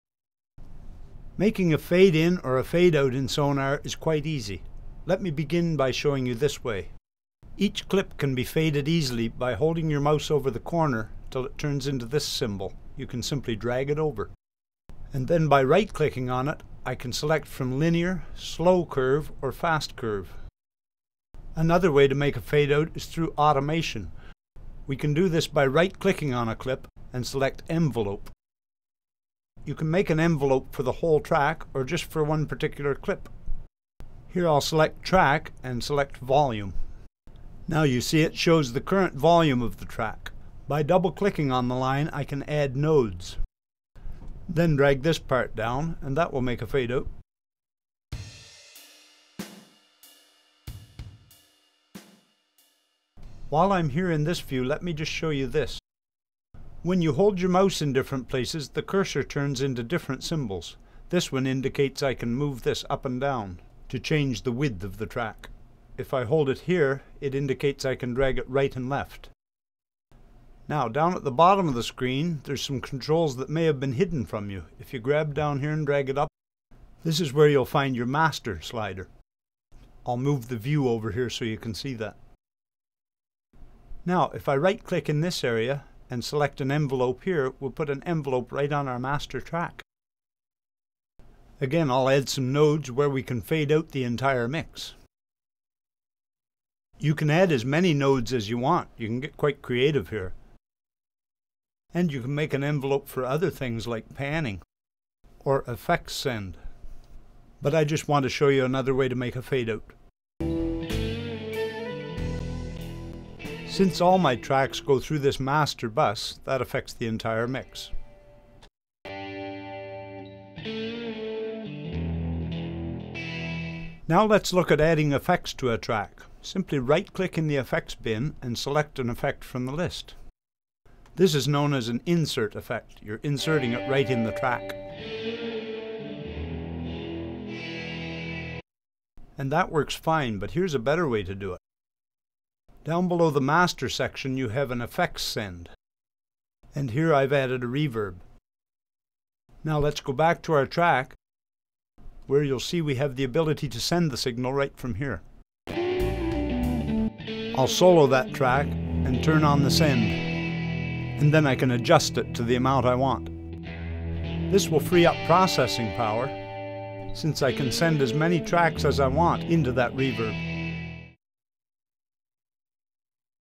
Sonar Fades and FX
Here we take a look at a couple ways to make a fade out. Also see 2 different ways to apply reverb to a track. Show 7